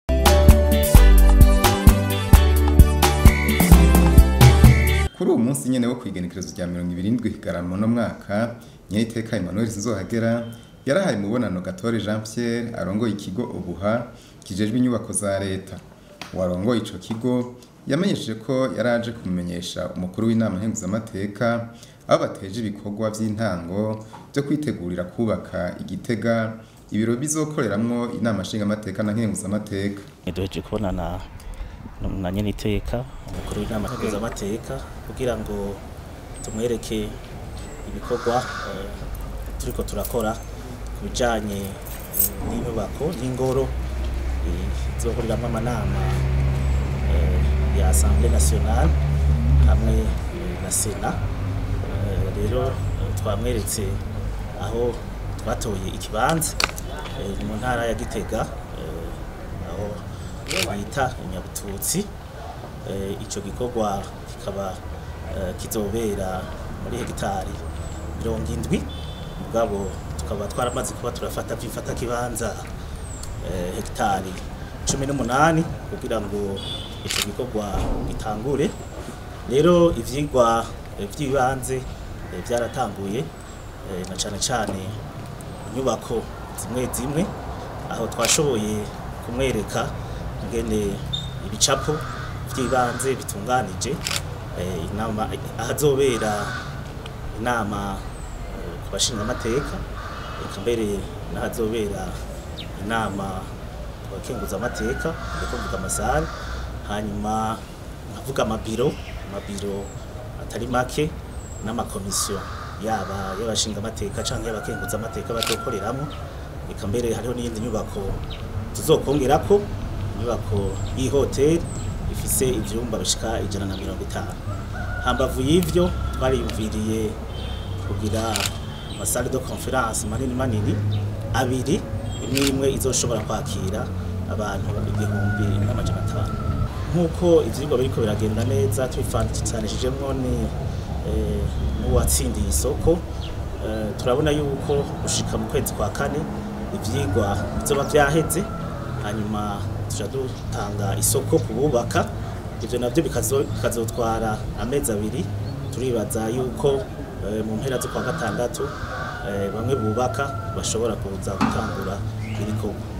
Ikigo OBUHA caramenyesheje Inama Nkenguzamateka aho giteje ibikorwa kwubaka ahazokorera urwo rwego
Kw’igenekerezo rya 27 Kigarama uno mwaka, Nyeniteka Emmanuel SINZOHAGERA yarahaye umubonano  GATORE Jean Pierre arongoye ikigo OBUHA kijejwe inyubako za Reta.

Uwo arongoye ico kigo yamenyesheje ko yari aje kumenyesha Umukuru w’Inama Nkenguzamateka aho bateje ibikorwa vy’intango vyo kwitegurira kwubaka i Gitega, Ibiro bizokoreramwo Inama Nshingamateka na Nkenguzamateka.